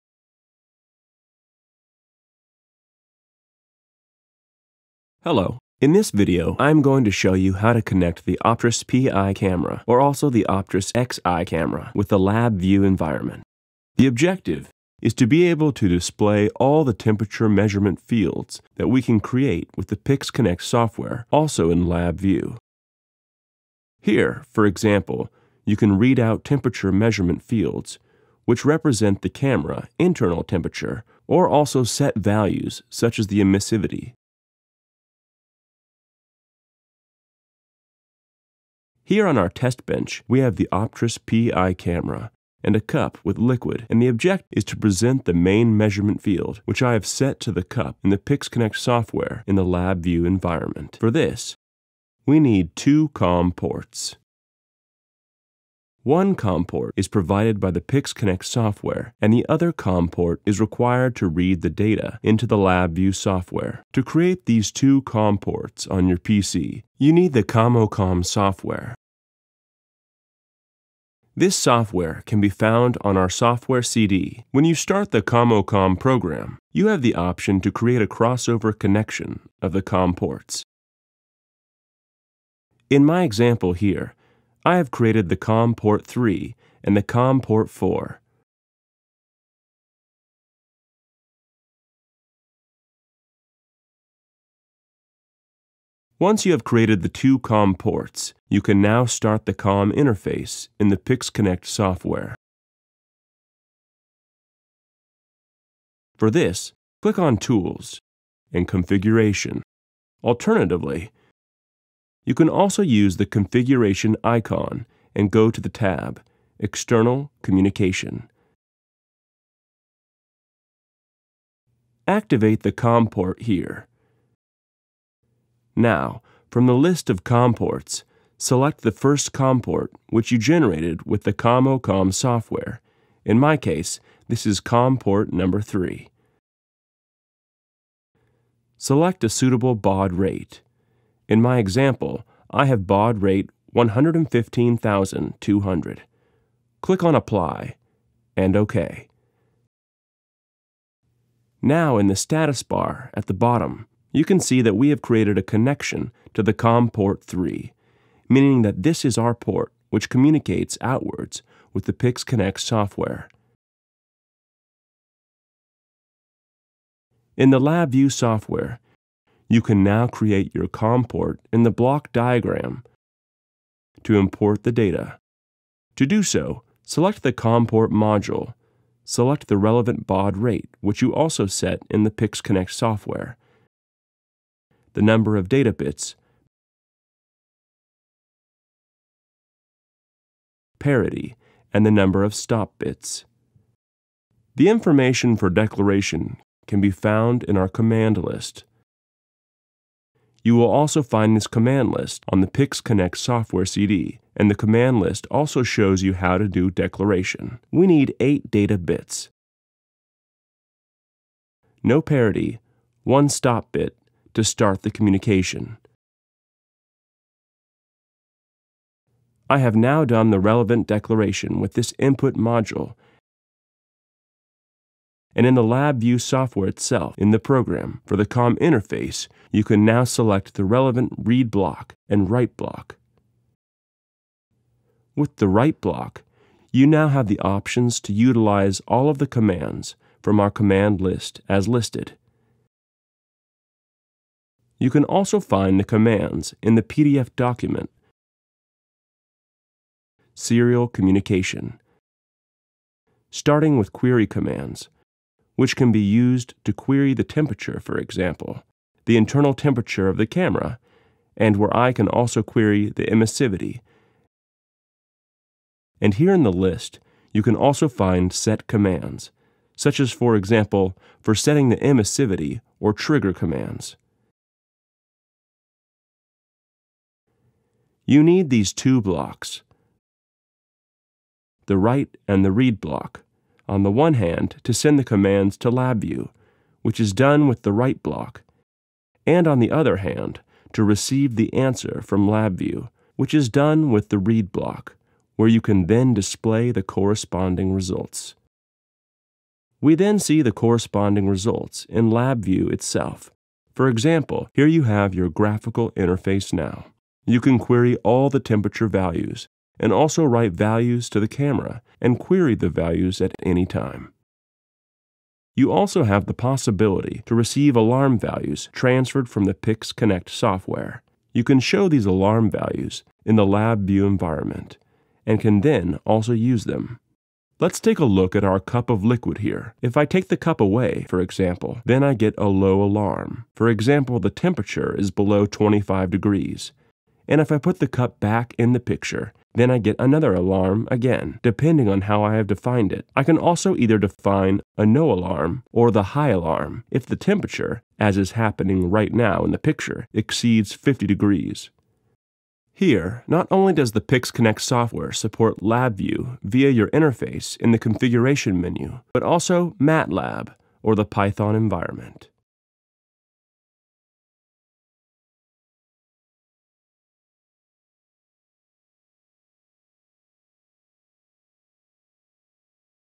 Optris PIX Connect: LabVIEW integration via virtual COM Ports
This video is a detailed guide on how to connect an Optris PI or XI camera to the LabVIEW environment using the PIX Connect software.

In this tutorial, you will learn:

The Purpose: How to read temperature measurement fields, internal camera temperature, and adjust settings like emissivity directly within LabVIEW.

Setup Requirements: A clear explanation of the hardware and software needed, including the use of com0com software to create virtual COM ports to facilitate communication between PIX Connect and LabVIEW.

Configuration: A step-by-step guide on how to configure both the PIX Connect and LabVIEW software to use the virtual COM ports and ensure a seamless data stream.

Functionality: See how to use read and write blocks in LabVIEW to send commands and receive data, allowing you to query and set values on the camera and receive alarm information.

This video is an essential resource for developers and engineers who want to integrate Optris camera data into their LabVIEW applications.